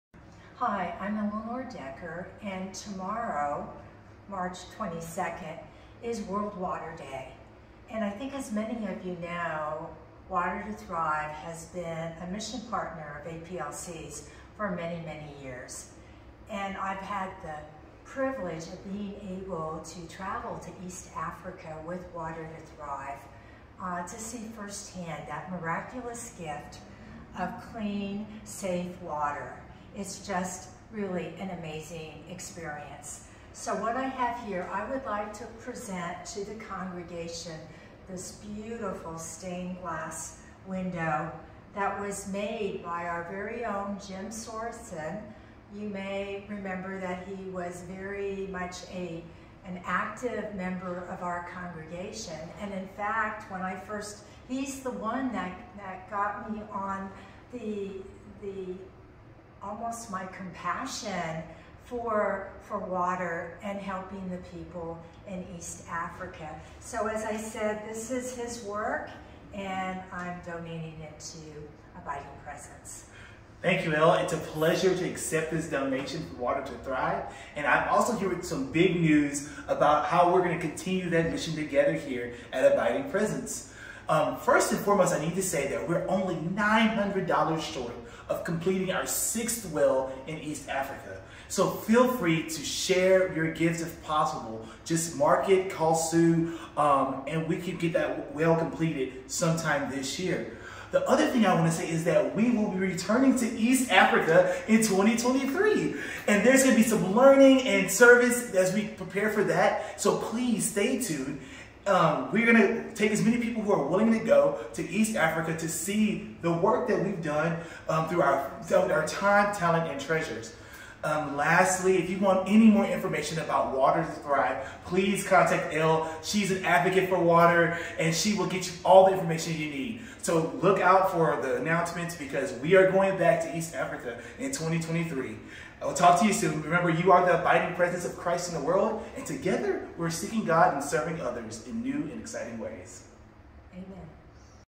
Celebrating World Water Day
Today is World Water Day and Elanore Gilbert Decker is here with a special presentation.